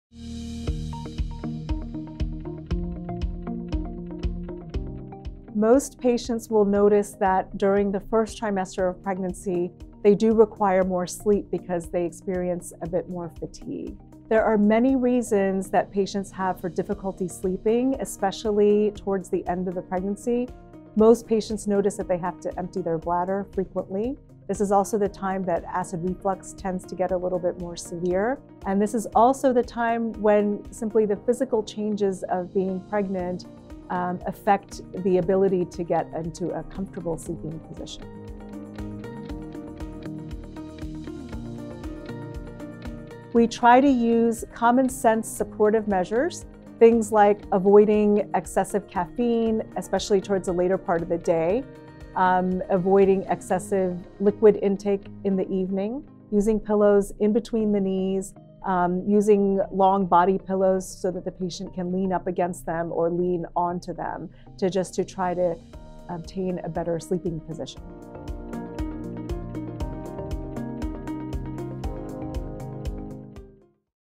How Pregnancy Affects Your Sleep
Dr. Janet Ko, Chief, Obstetrics and Gynecology at Einstein Medical Center Montgomery, describes how sleep patterns could change as you move through each trimester of pregnancy and offers helpful tips for a better night’s sleep.